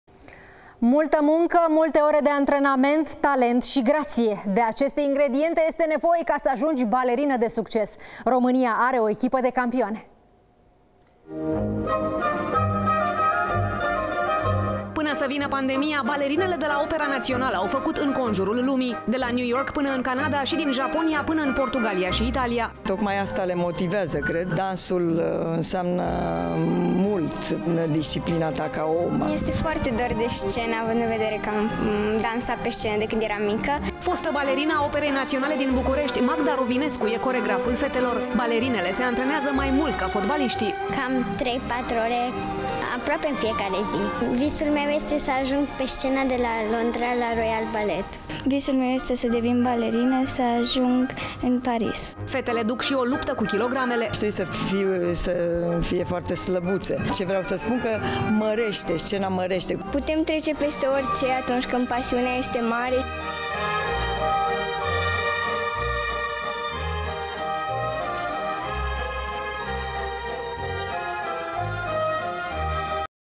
Pana sa vina pandemia, balerinele de la Opera Nationala au facut inconjurul lumii!
Multă muncă, multe ore de antrenament, talent și grație! De aceste ingrediente este nevoie ca să ajungi balerină de succes. România are o echipă de campioane!